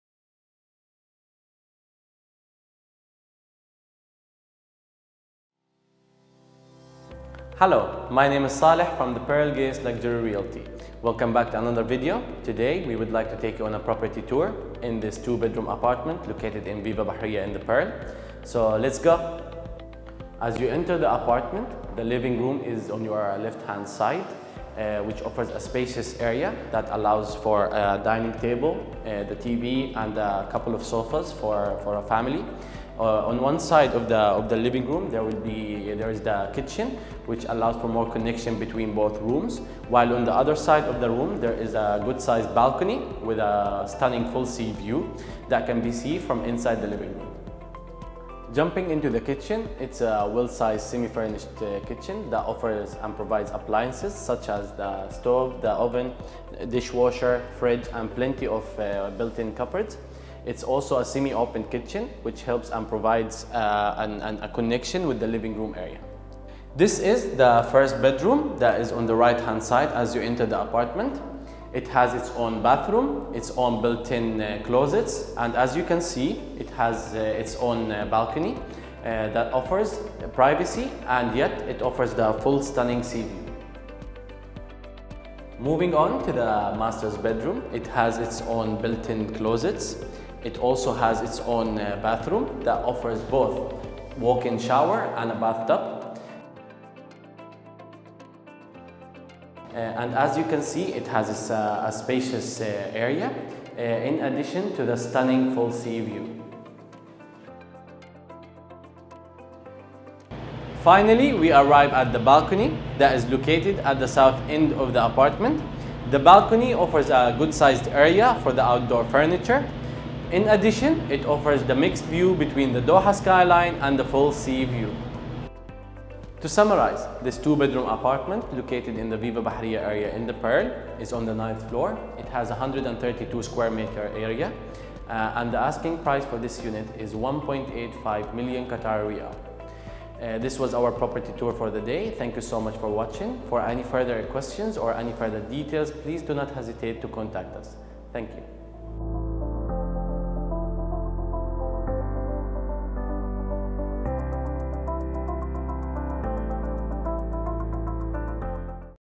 2 BR Apartment with Sea view in The Pearl for Sale 1.85 Million QAR | The Pearl Gates
Sale price:  1,850,000 QAR
Bedroom :  2
Bathroom: 3
Area :  1421 SQFT/ 132sqm
Location : Viva Bahriya, The Pearl